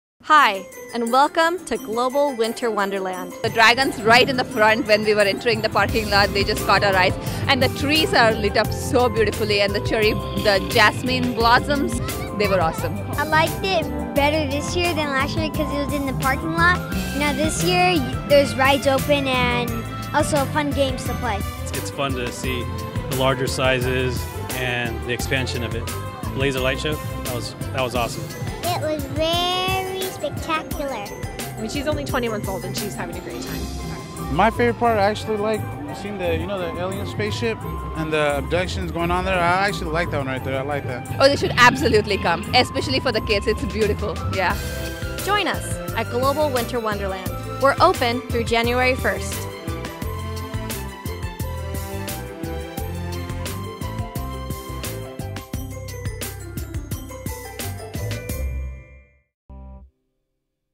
Global Winter Wonderland - Guests Say It Best
The Magical Journey continues this year at Global Winter Wonderland. In this short video, GWW Correspondent, Regan Eymann, talks to customers around the Park to get their reactions to this year's Festival now that it's inside California's Great America.